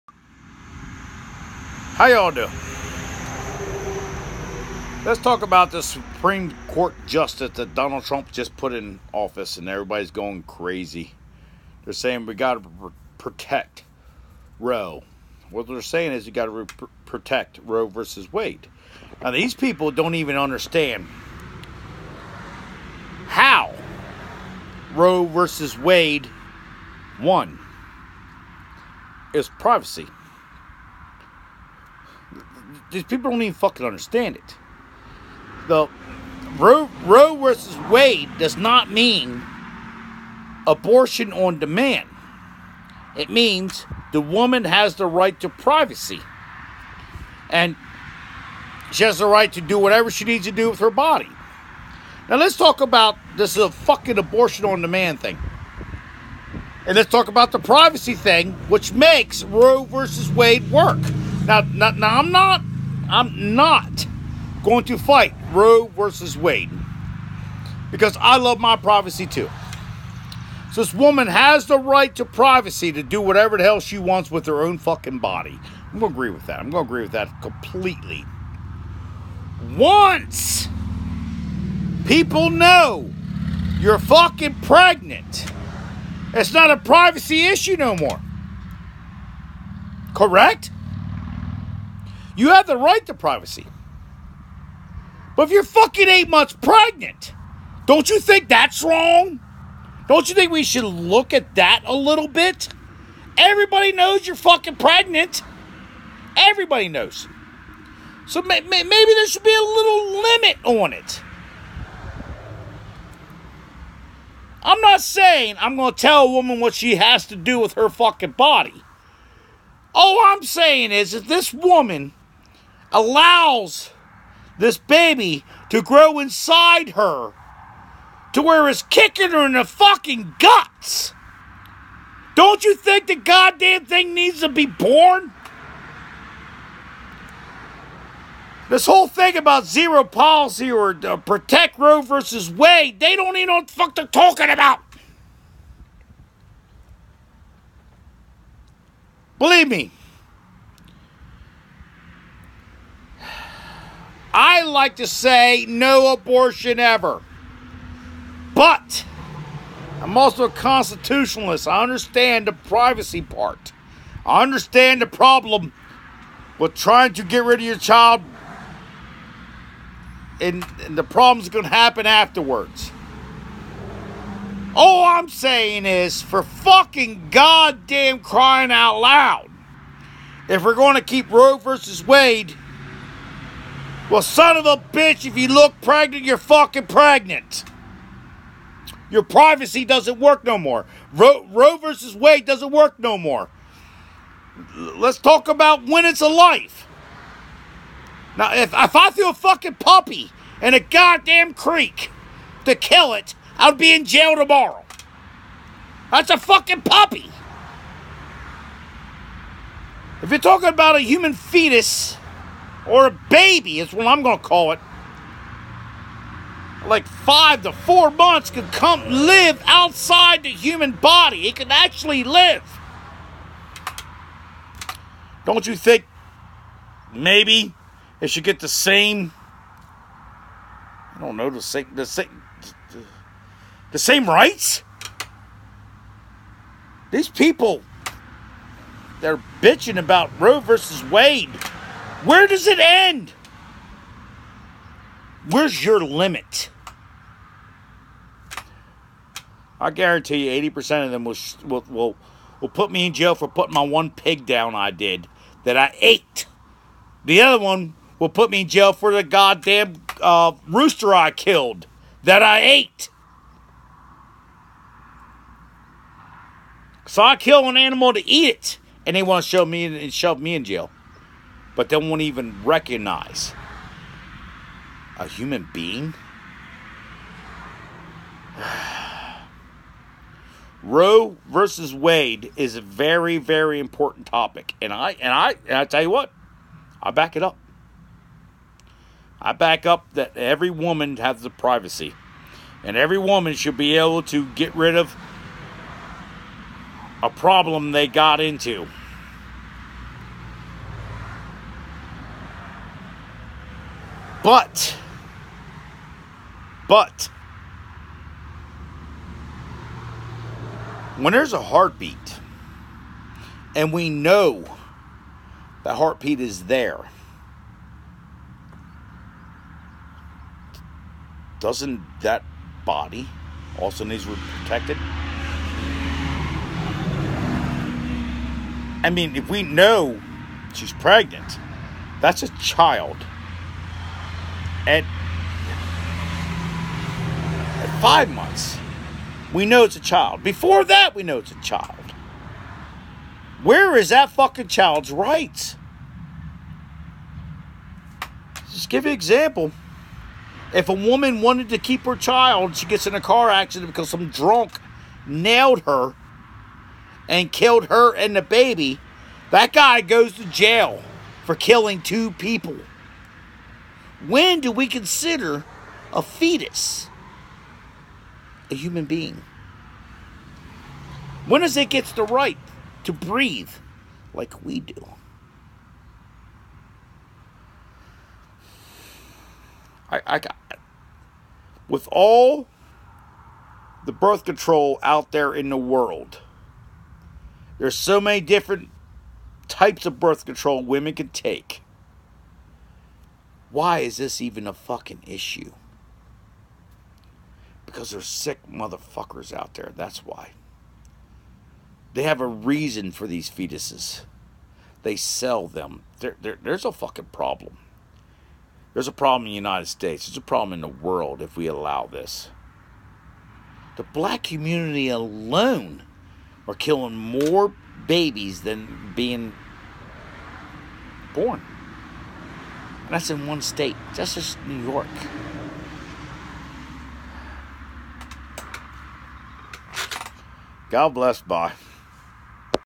Supreme court justice the new one
It’s hard to believe the left going so fucking crazy because of the supreme court justice. That’s because they like doing things illegally unconstitutional. The Supreme Court has no power. These people are acting like they have all the power